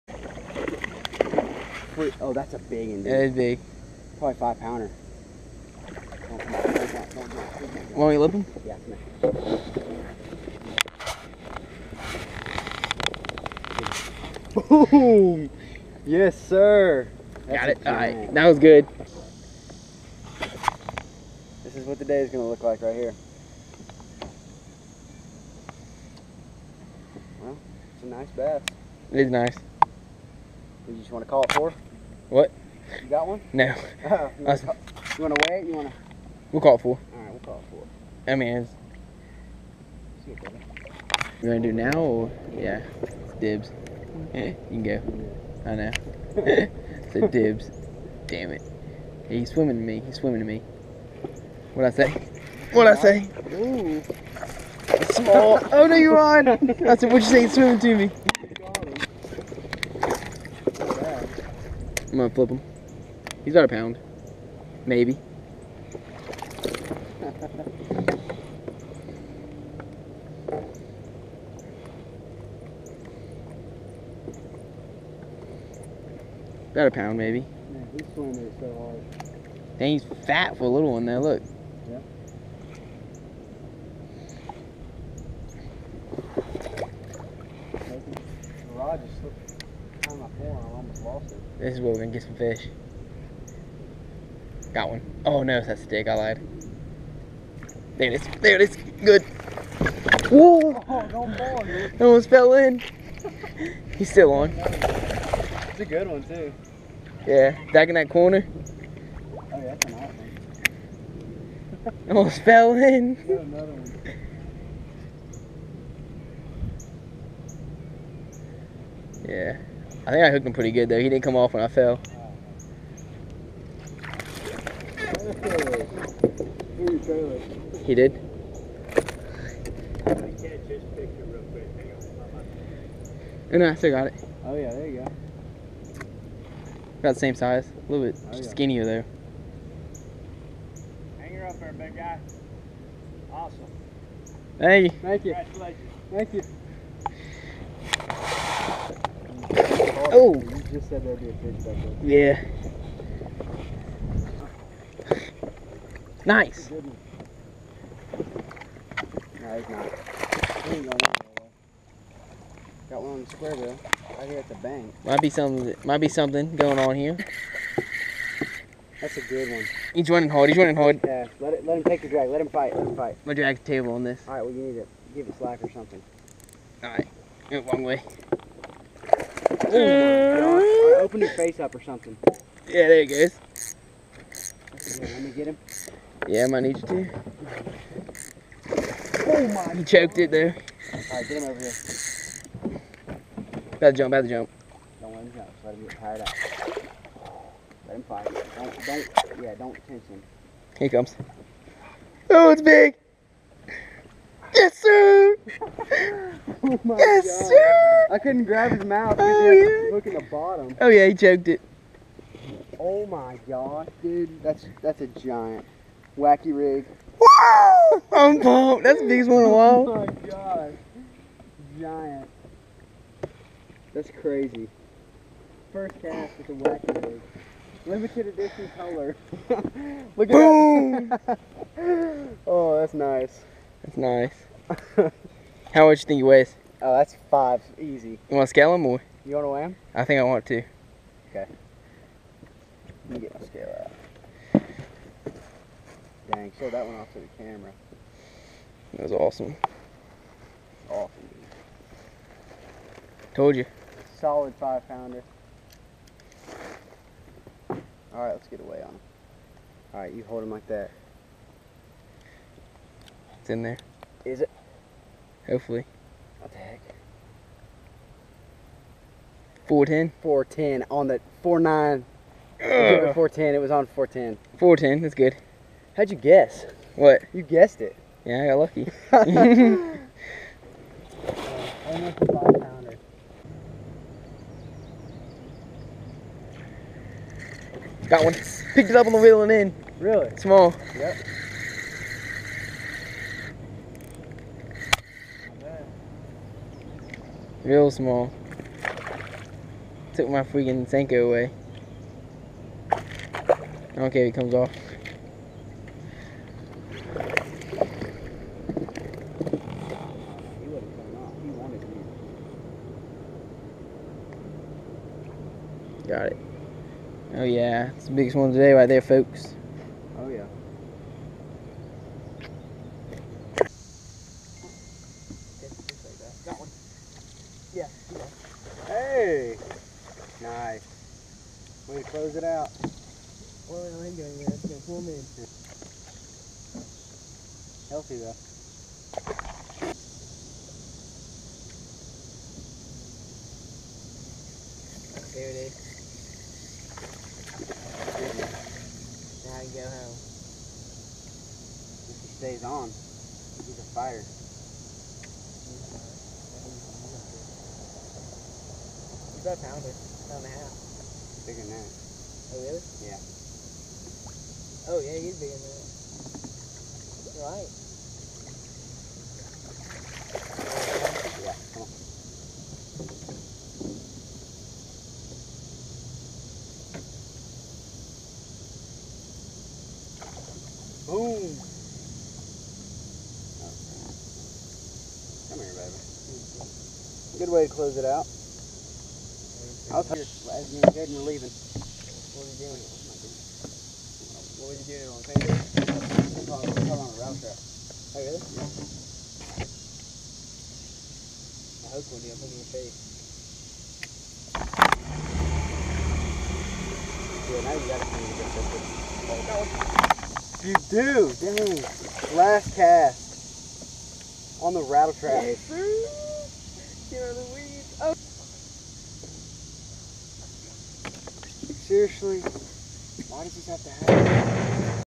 Jonah and Justin Fishing
caught a ton of fish on the water today mostly on jigs and on wacky rigs! Went fishing Jonah from Jonah and the fish YouTube channel!